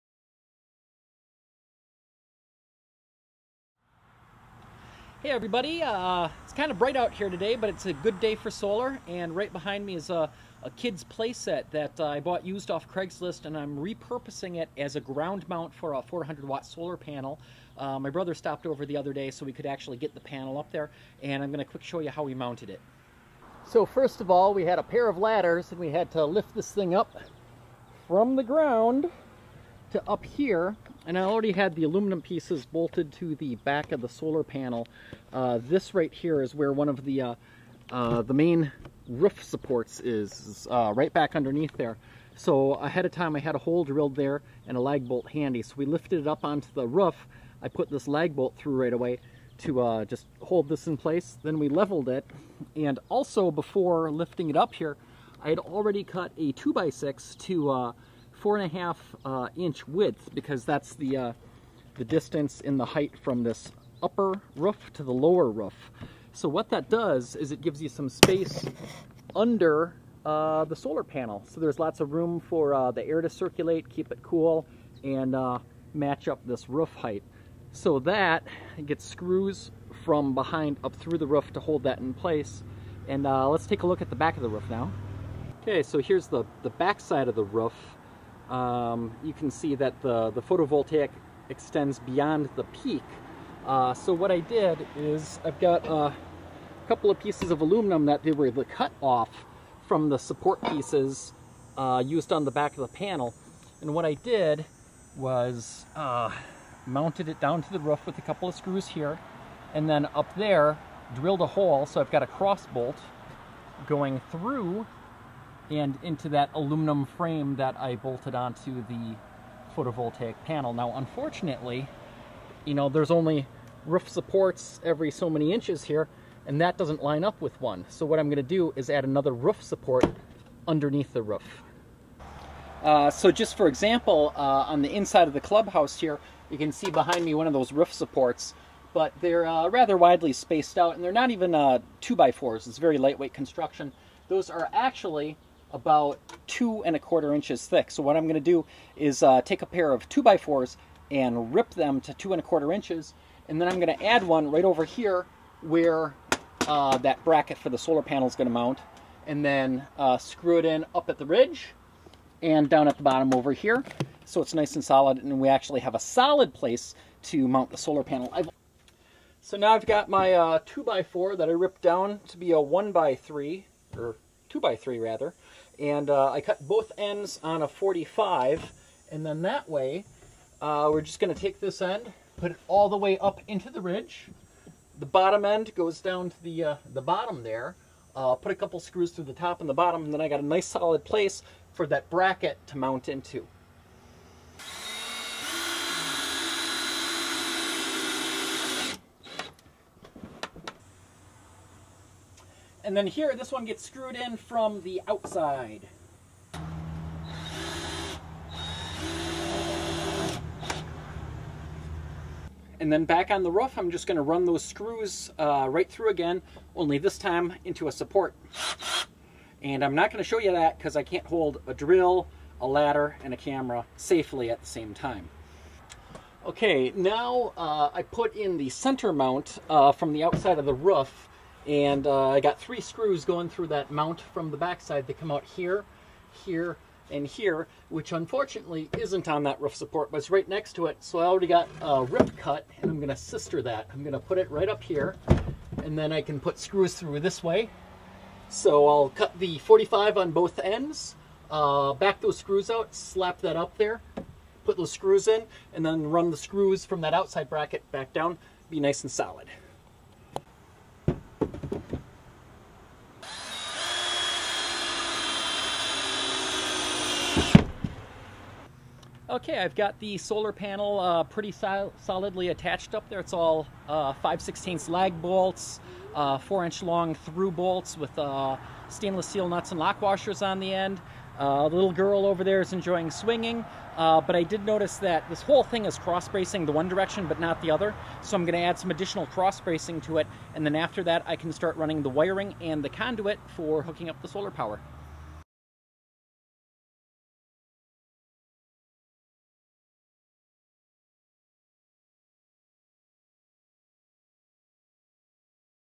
Mounting PV to the Solar Swing-Set
I get the 405-watt 48V solar panel attached to the ground-mount - a repurposed children's swing-set and playhouse.